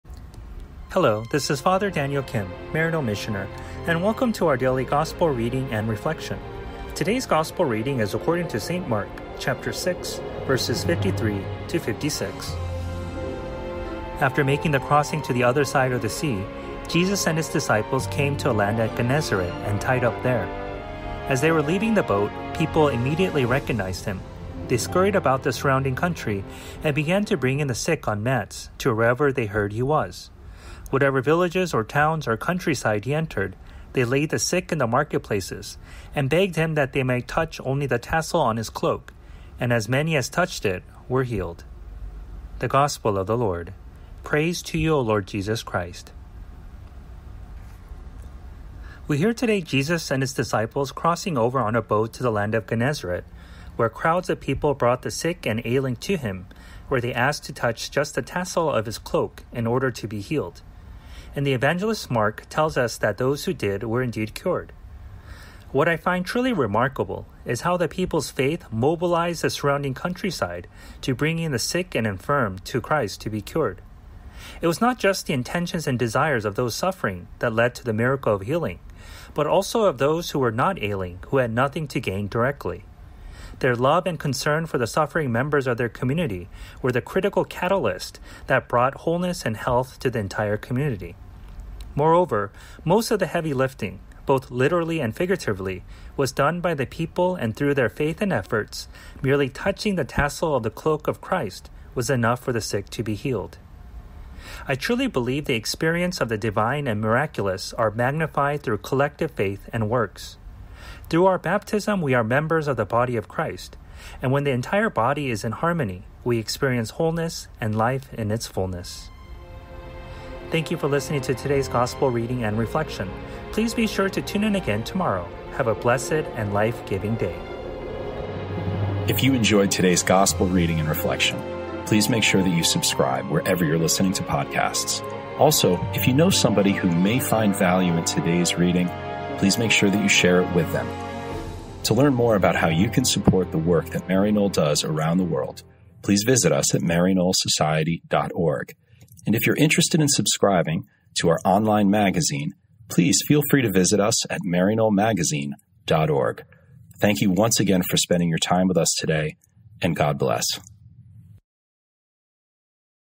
Mark 6:53-56, Daily Gospel Reading and Reflection | Maryknoll Fathers and Brothers
Join Fr. Daniel Kim, M.M on Monday of the Fifth Week in Ordinary Time, 2022, for today's audio reading and reflection

Whatever villages or towns or countryside he entered,
they laid the sick in the marketplaces and begged him that they might touch only the tassel on his cloak;and as many as touched it were healed.